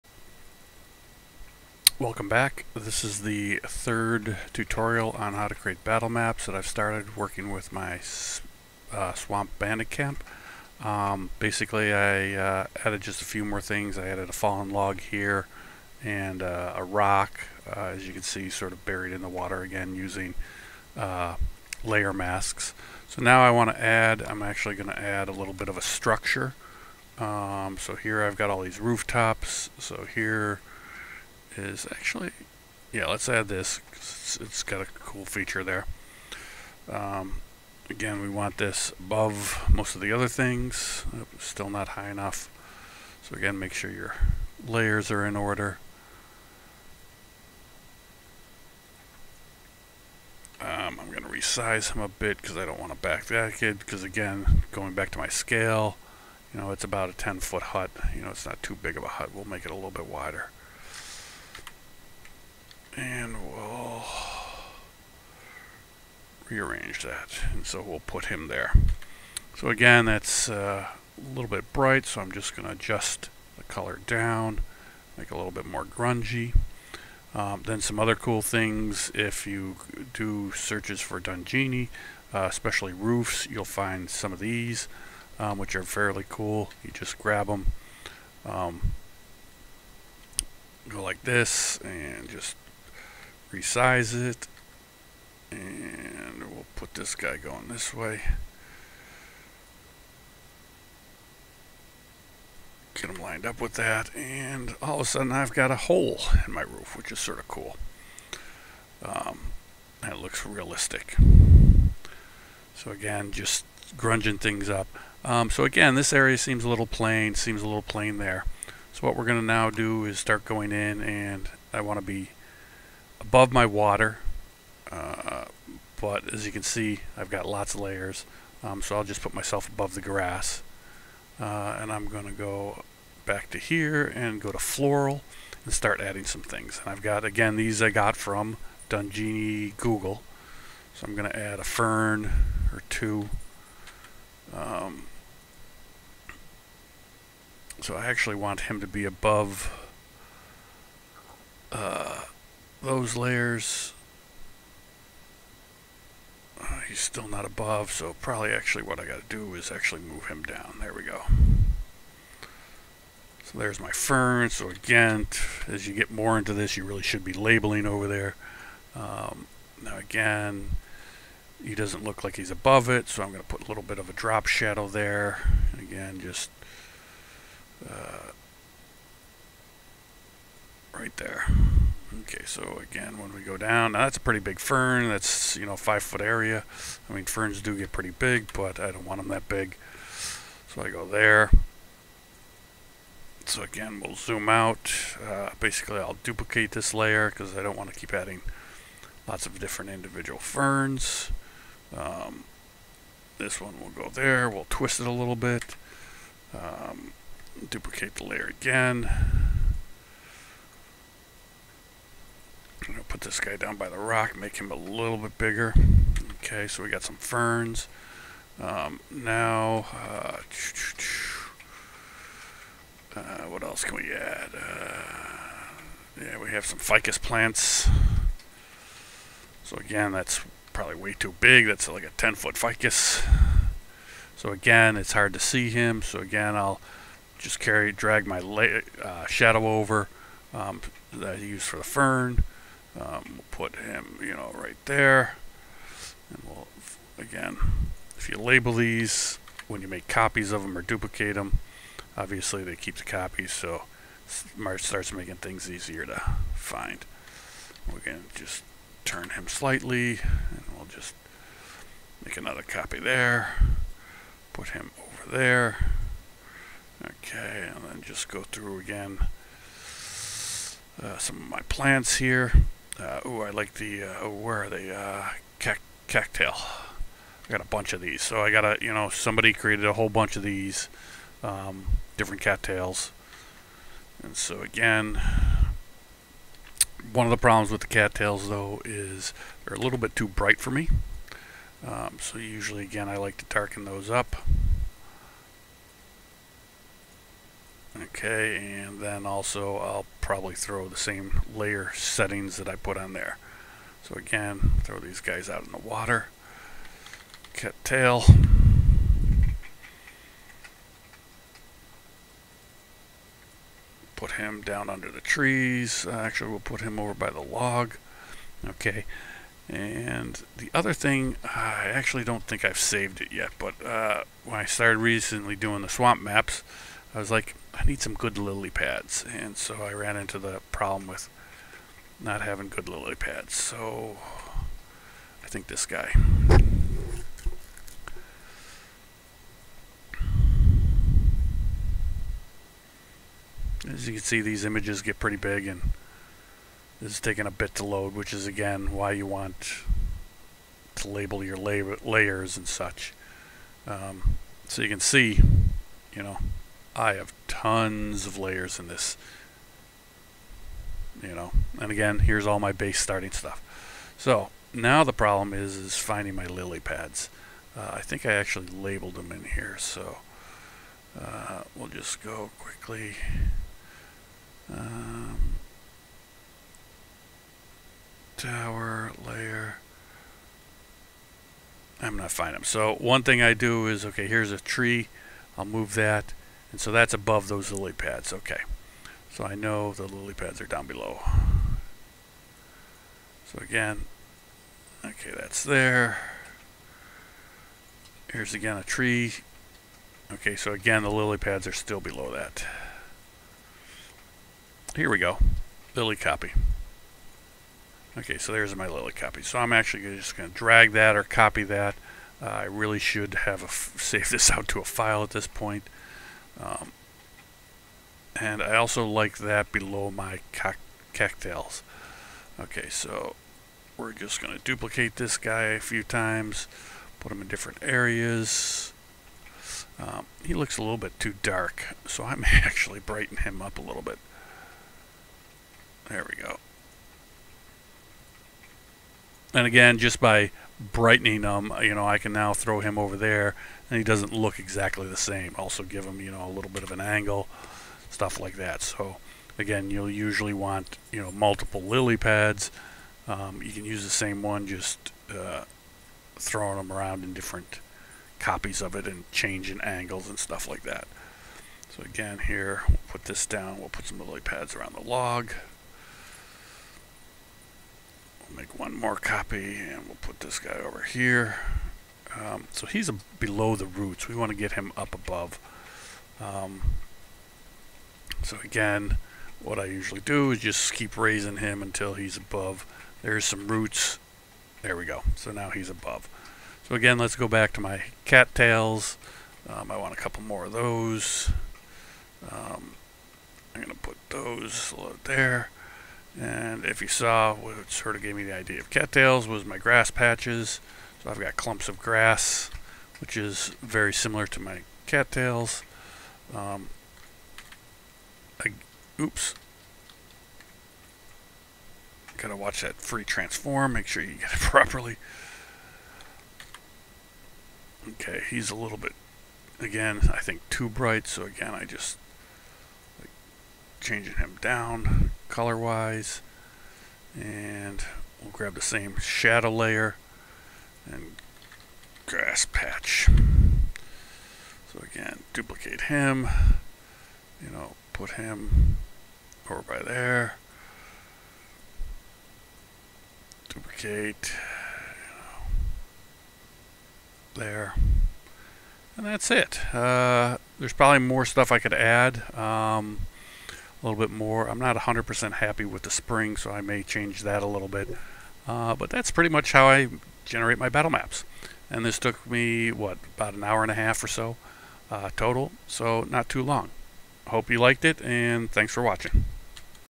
BattleMapTutorial Step3
Basic Tutorial using photoshop to create realistic battlemaps for gaming. Step 3 of 3. Final Map